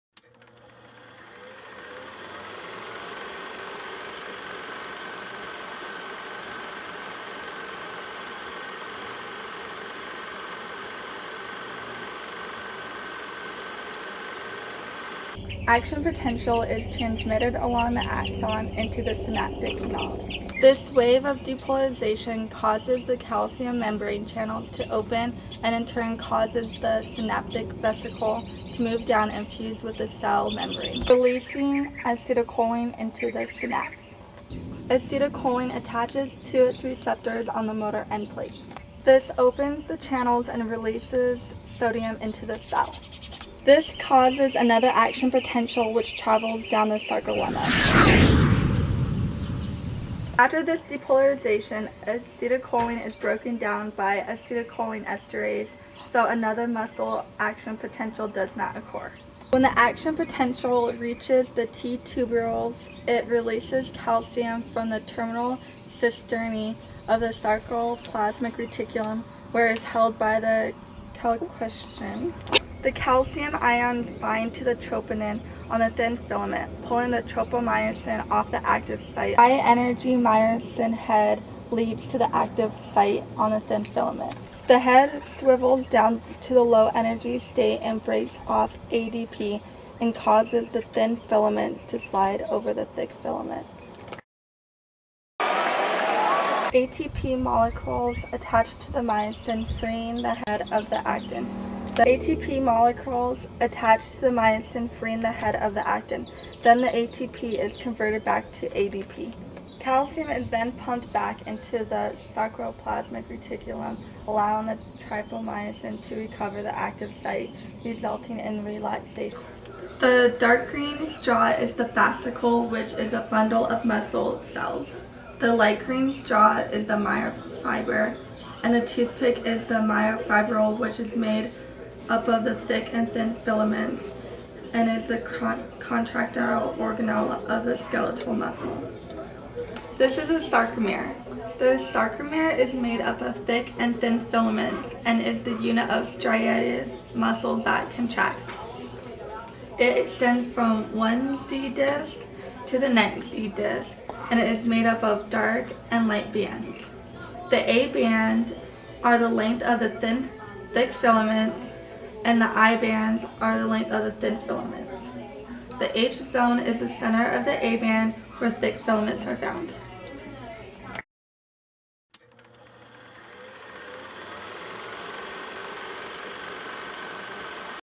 Muscle Contraction
Amber & Korey present the physiology of muscle contraction to their BIO 201 course at Scottsdale Community College